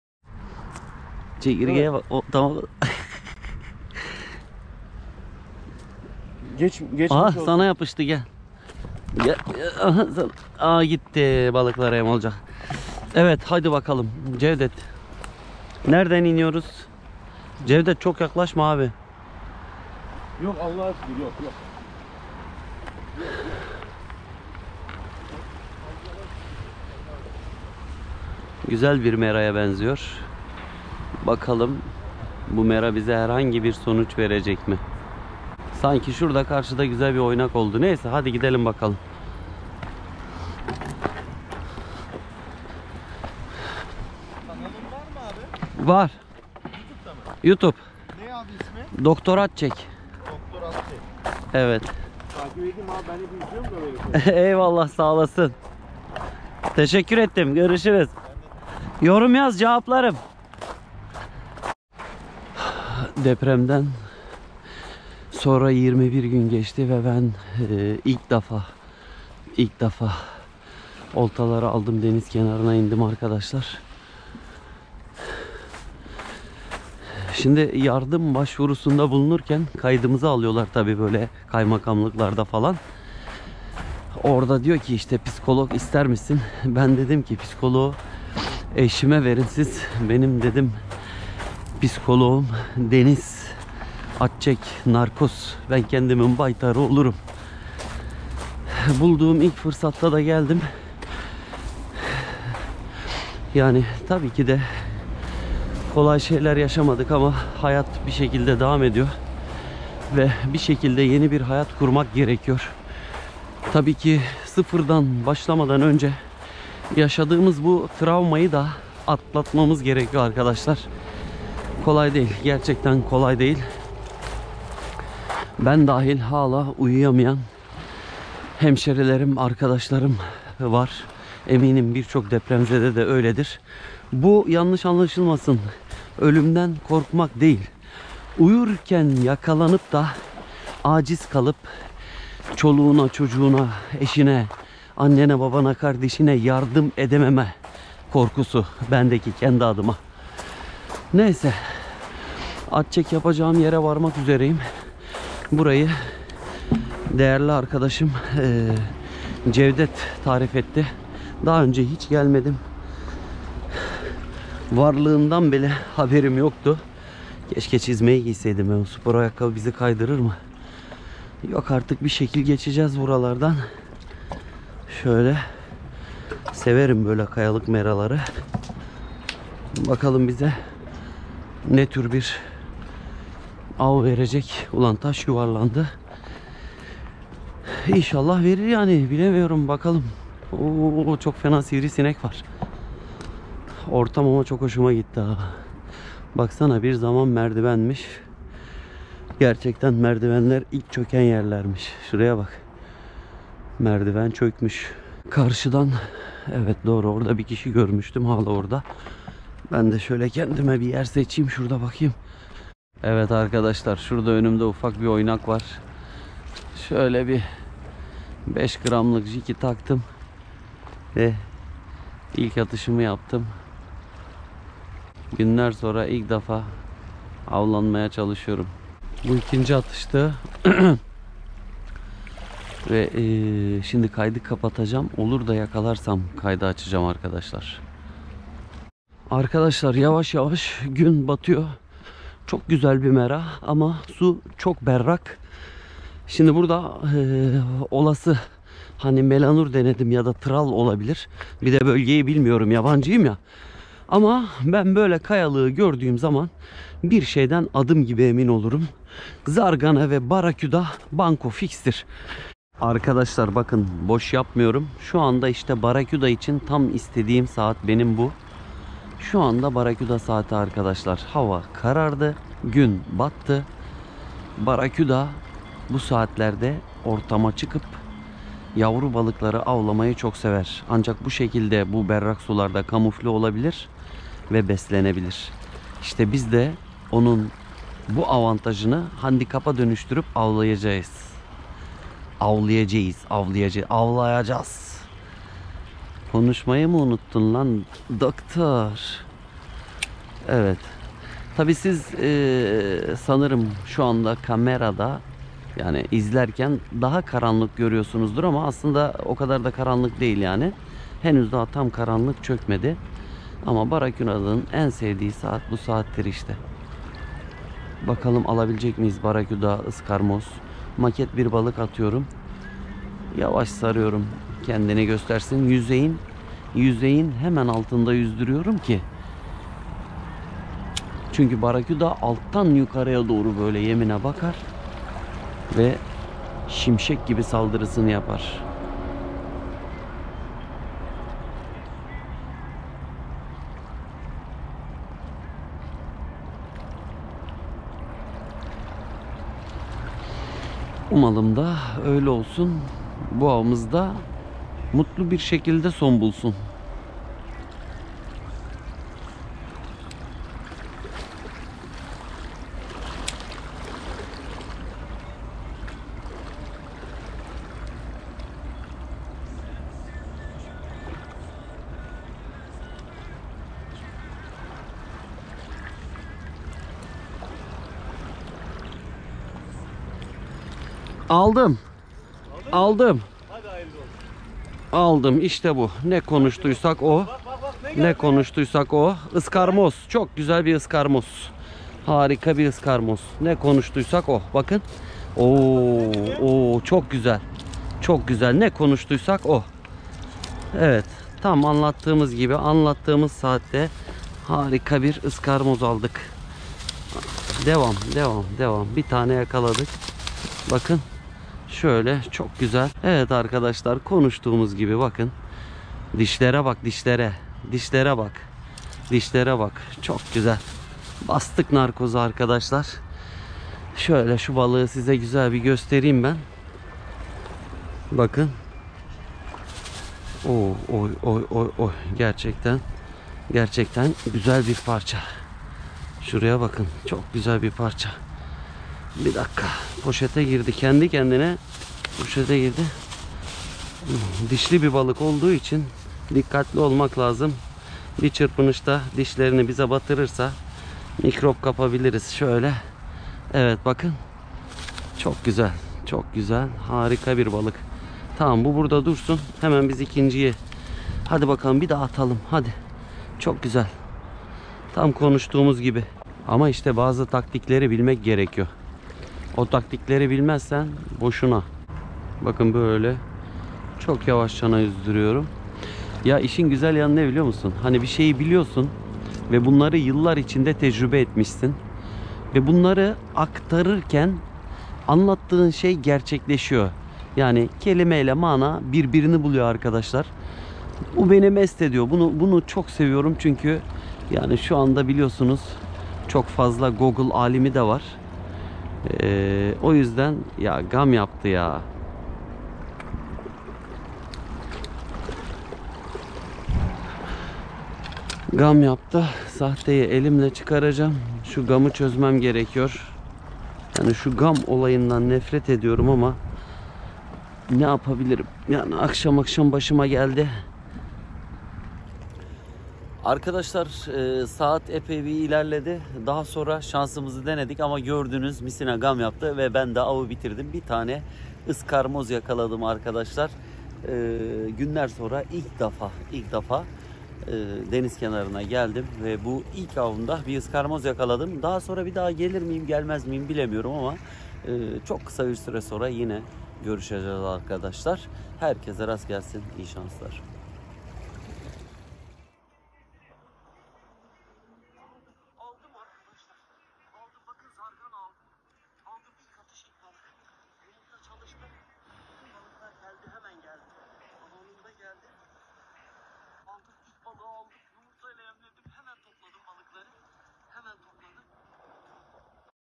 İlk defa gittiğin mera da balık yakalamayı öğren // Bazı taktikleri bilirsen BALIK YAKALARSIN
İzle uygula sabırlı ol yakala. Bilgileri verirken yakaladım